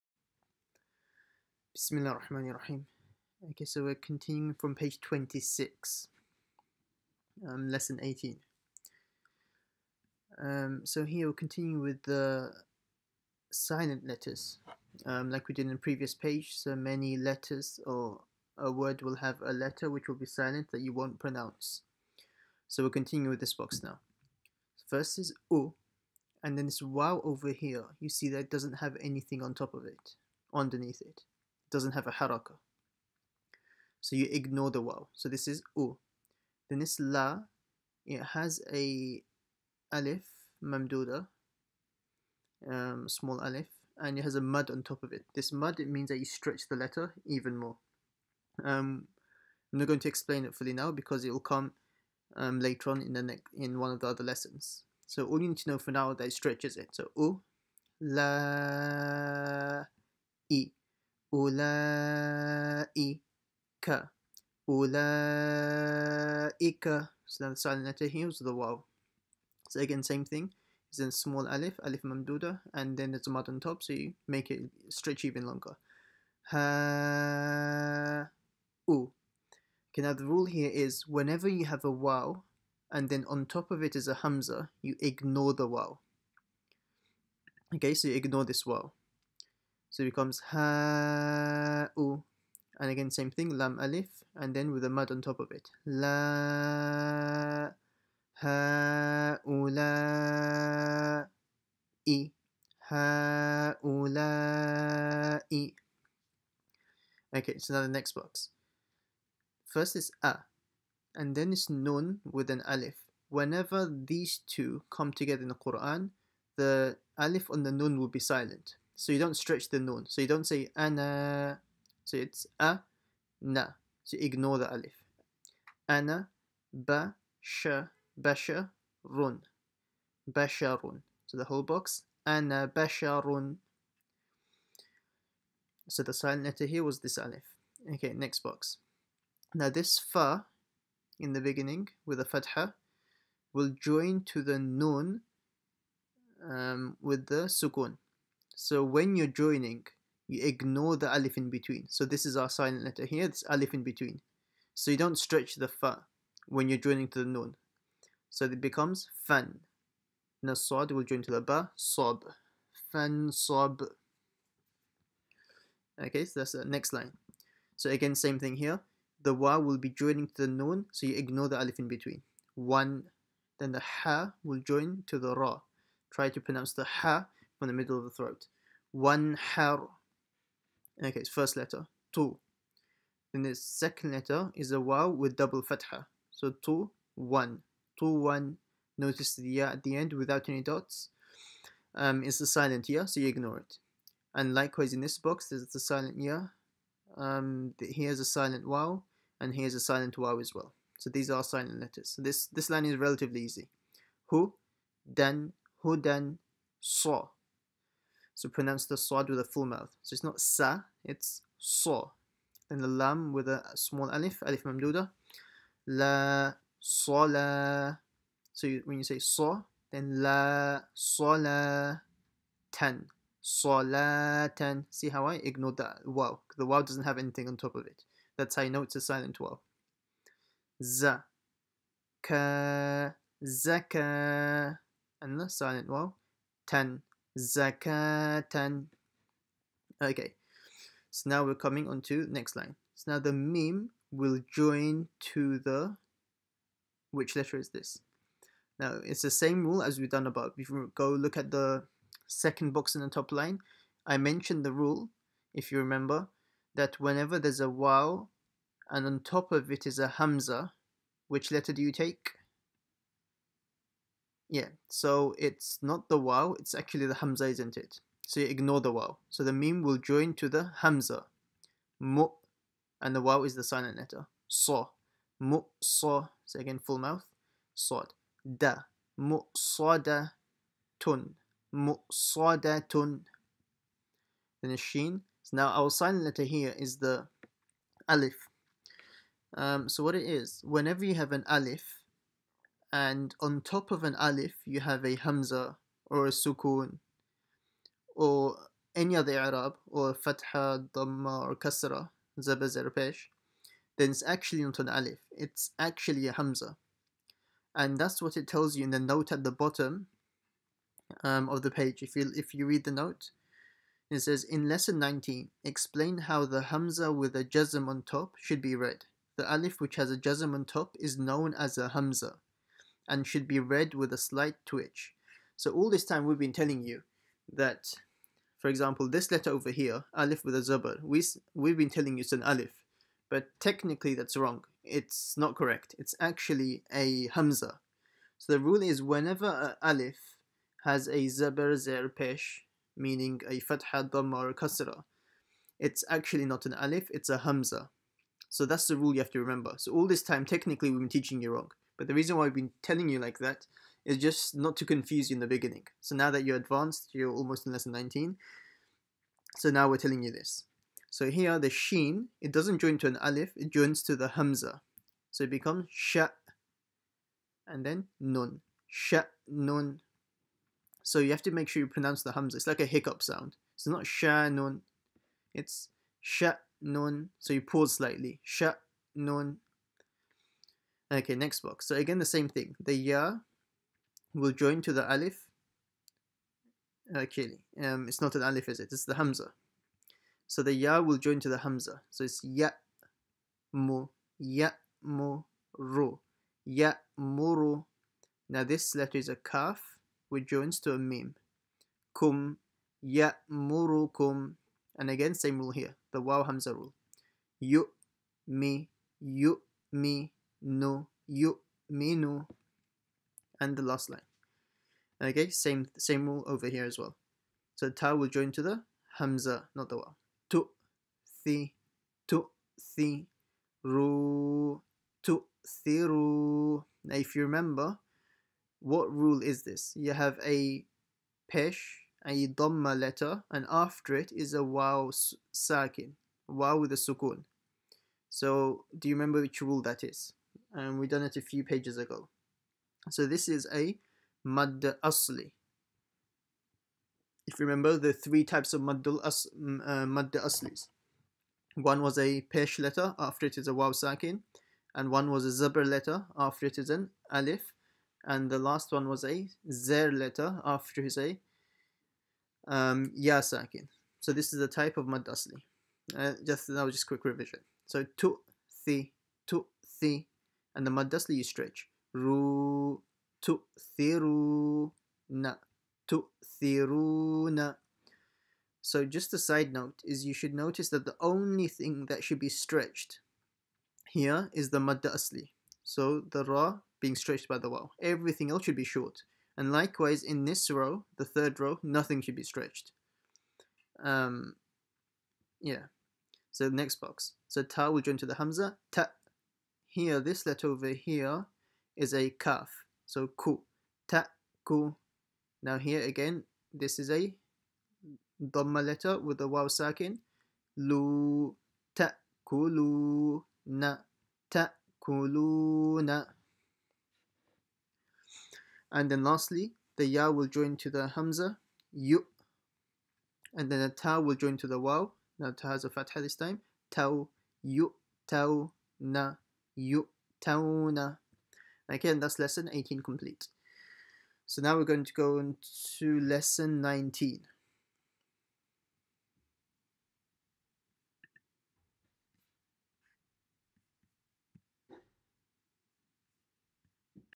Ahsan al-Qawa'id - Lesson 18 Page 26 PART 2
Continuing from lesson 18.

This lesson we learn about those letters that should not be read or pronounced.

Secondly, we learn how to pronounce a hamza.

And lastly, we will learn that an alif which has a sukoon/ jazm on top is known as a hamza and should be read with a slight twitch.